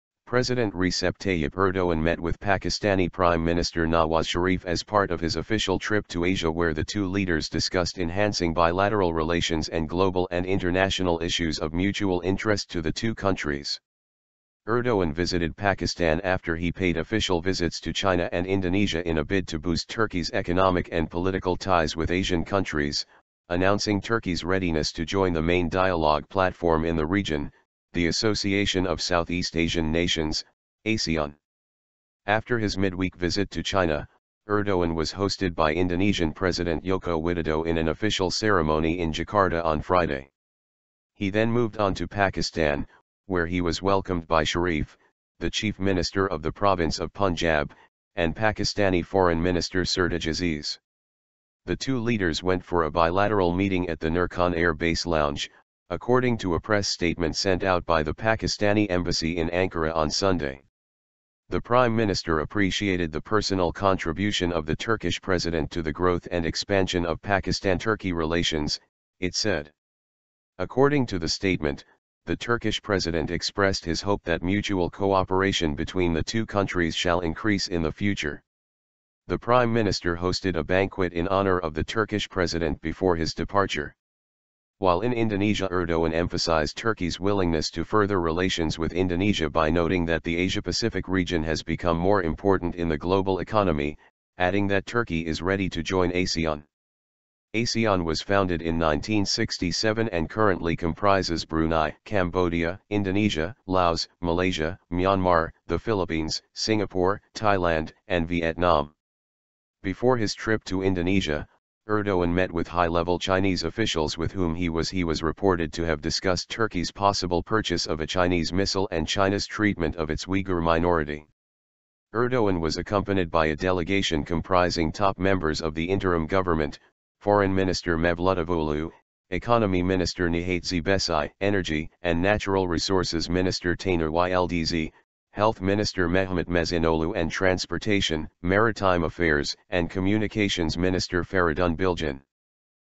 Erdoğan visits Pakistan as part of official Asia tour to boost ties
President Recep Tayyip Erdoğan met with Pakistani Prime Minister Nawaz Sharif as part of his official trip to Asia where the two leaders discussed enhancing bilateral relations and global and international issues of mutual interest to the two countries.

Erdoğan visited Pakistan after he paid official visits to China and Indonesia in a bid to boost Turkey's economic and political ties with Asian countries, announcing Turkey's readiness to join the main dialogue platform in the region, the Association of Southeast Asian Nations (ASEAN).

After his mid-week visit to China, Erdoğan was hosted by Indonesian President Joko Widodo in an official ceremony in Jakarta on Friday. He then moved on to Pakistan, where he was welcomed by Sharif, the chief minister of the province of Punjab, and Pakistani Foreign Minister Sertaj Aziz.

“The two leaders went for a bilateral meeting at the Nur Khan Air Base lounge,” according to a press statement sent out by the Pakistani Embassy in Ankara on Sunday.

“The Prime Minister appreciated the personal contribution of the Turkish President to the growth and expansion of Pakistan-Turkey relations,” it said.

According to the statement, “The Turkish President expressed his hope that mutual cooperation between the two countries shall increase in the future. The Prime Minister hosted a banquet in honour of the Turkish President before his departure.”

While in Indonesia Erdoğan emphasized Turkey's willingness to further relations with Indonesia by noting that the Asia-Pacific region has become more important in the global economy, adding that Turkey is ready to join ASEAN.

ASEAN was founded in 1967 and currently comprises Brunei, Cambodia, Indonesia, Laos, Malaysia, Myanmar, the Philippines, Singapore, Thailand and Vietnam.

Before his trip to Indonesia, Erdoğan met with high-level Chinese officials with whom he was he was reported to have discussed Turkey's possible purchase of a Chinese missile and China's treatment of its Uighur minority.

Erdoğan was accompanied by a delegation comprising top members of the interim government: Foreign Minister Mevlüt Çavuşoğlu, Economy Minister Nihat Zeybekci, Energy and Natural Resources Minister Taner Yıldız, Health Minister Mehmet Müezzinoğlu and Transportation, Maritime Affairs and Communications Minister Feridun Bilgin.